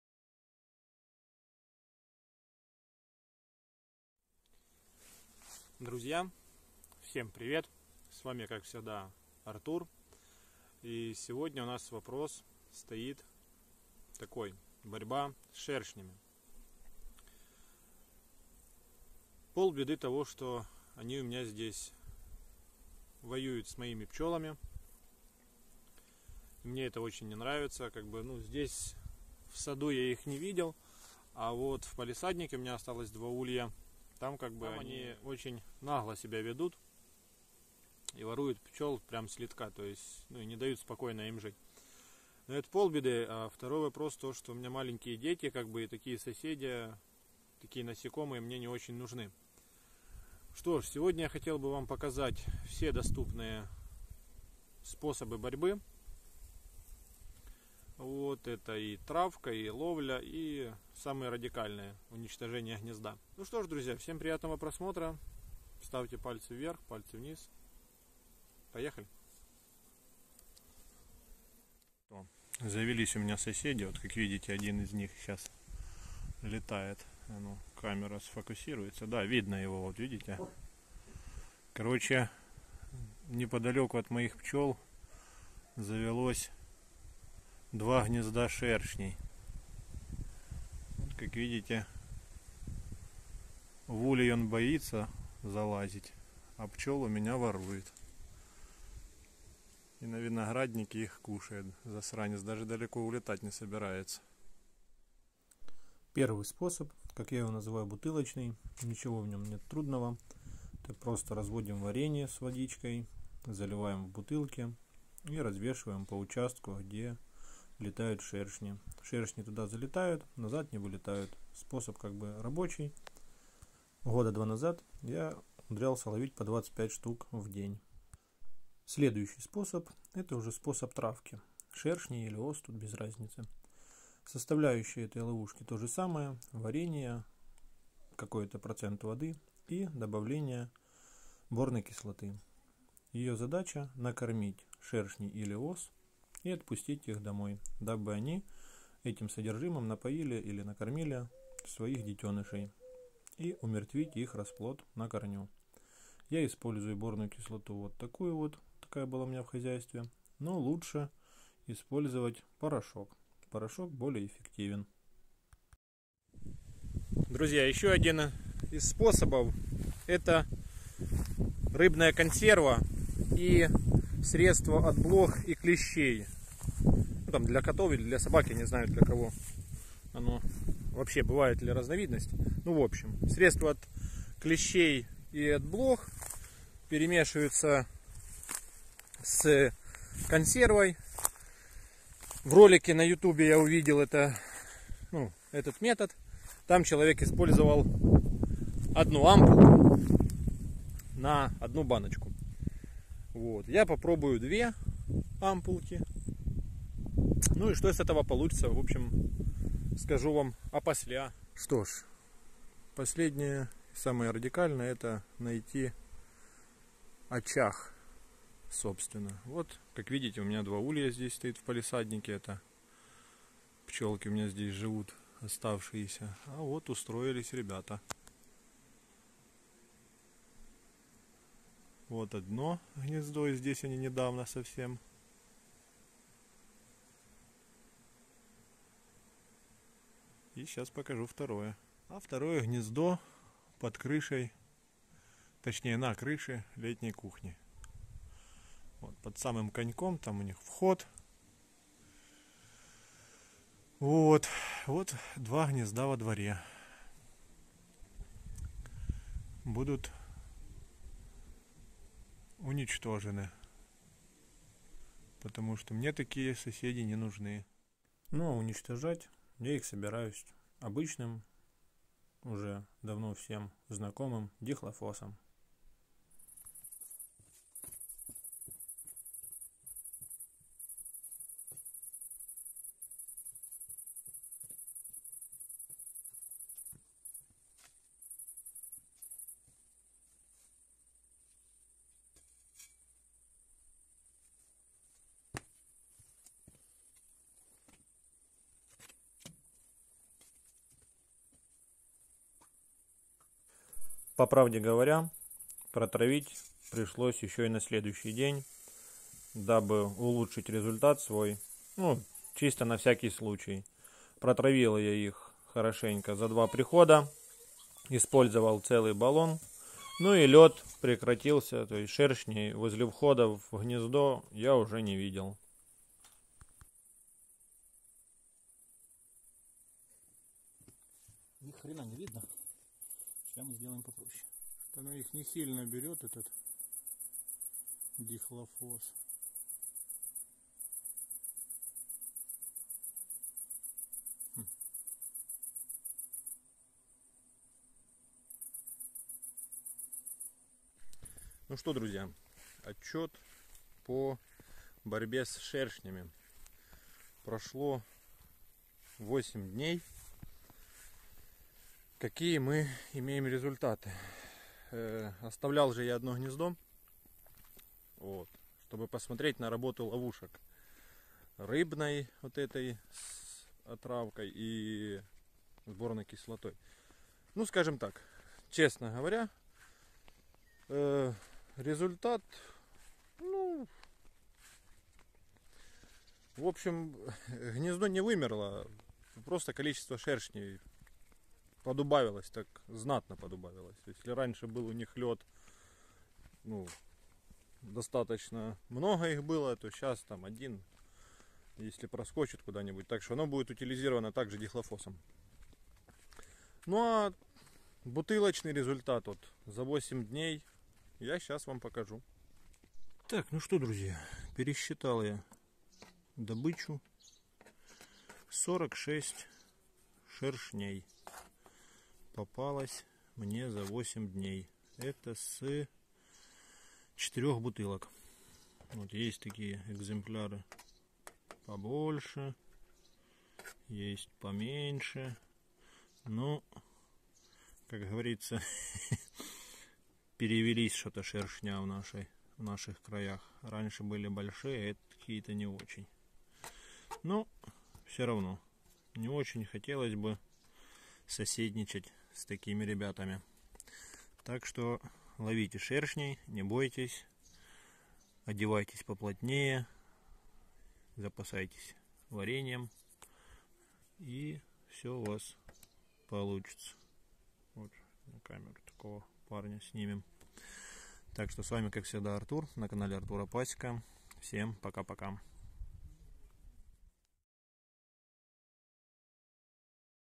4 СПОСОБА БОРЬБЫ С ШЕРШНЯМИ. Как избавится от шершней? АРТУРА&ПАСЕКА.
Доброго времени суток дорогие друзья и гости моего канала👋😊Решил поделится с Вами опытом борьбы с такими не совсем желаемыми соседями...опыт считаю хороший потому как все увенчалось успехом, и потому решил поделится с Вами.